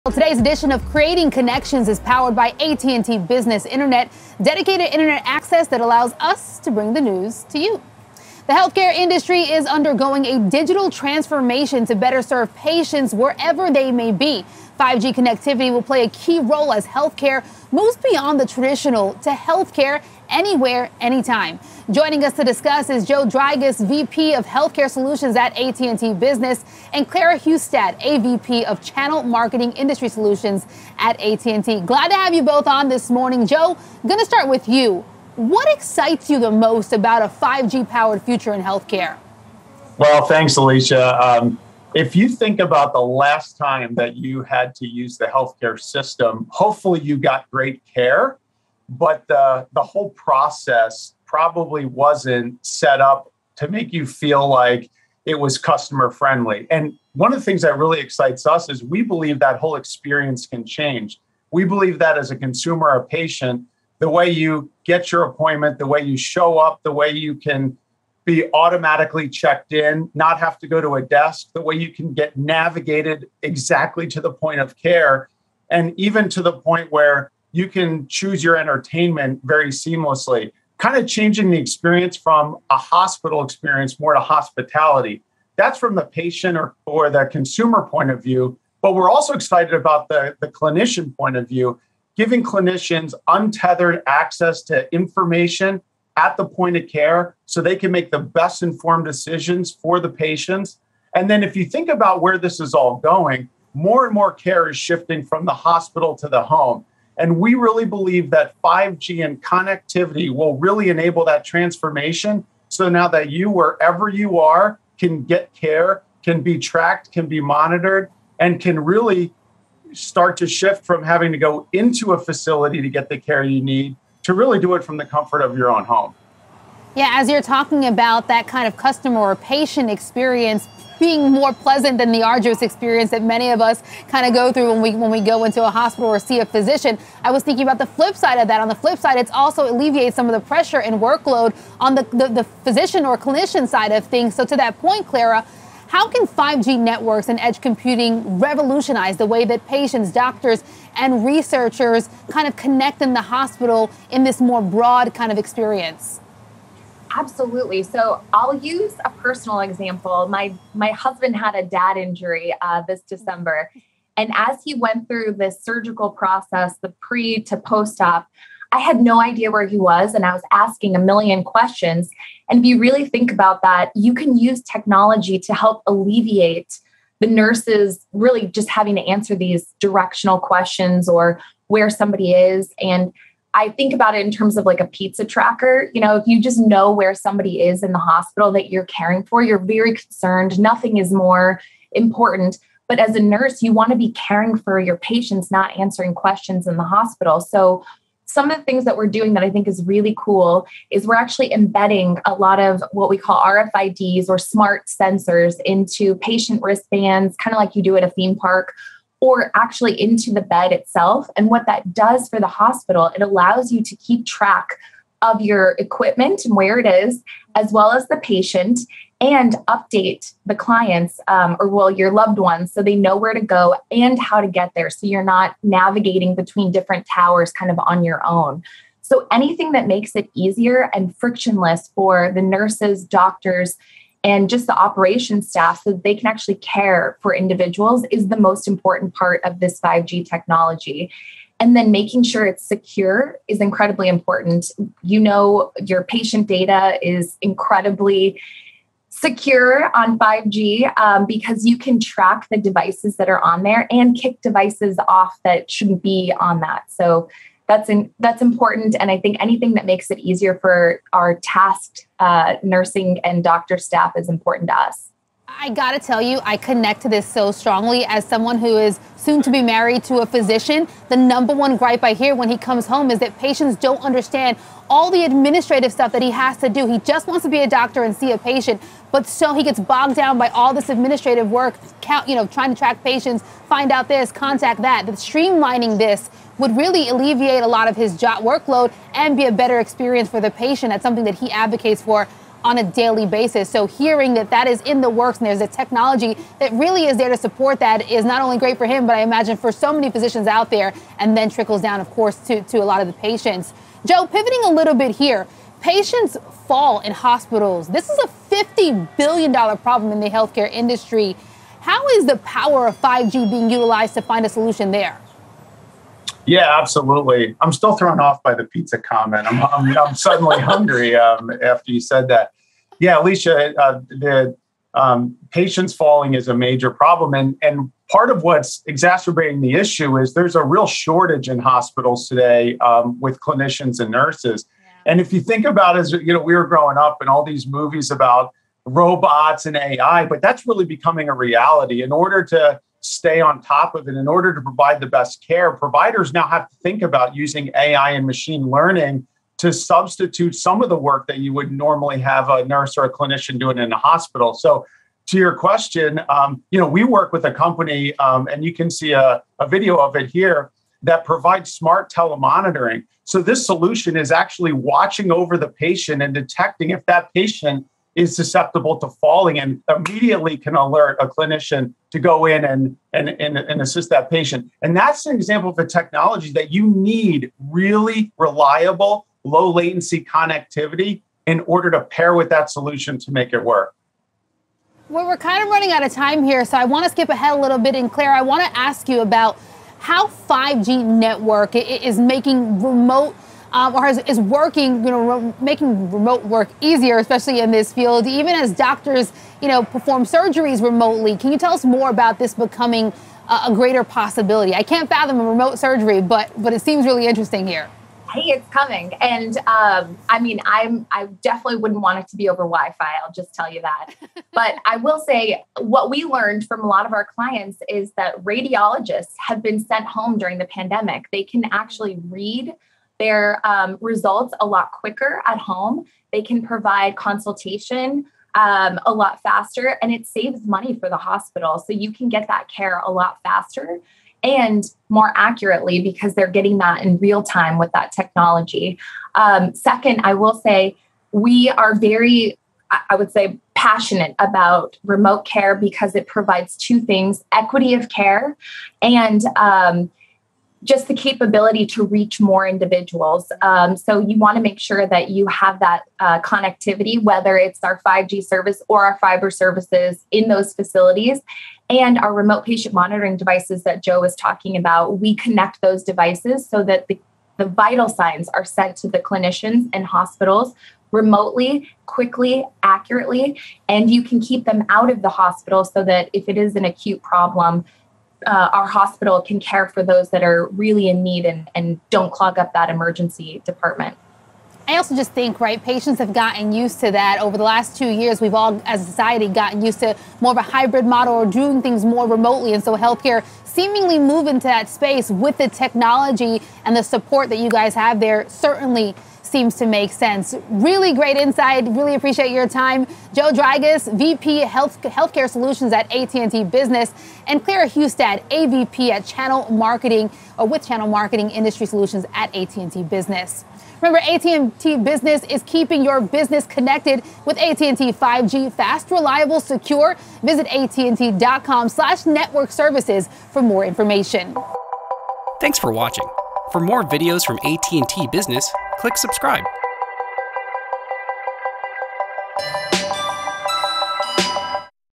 How can 5G impact healthcare?
What are some tangible ways that 5G is impacting health care? Presented by @ATTBusiness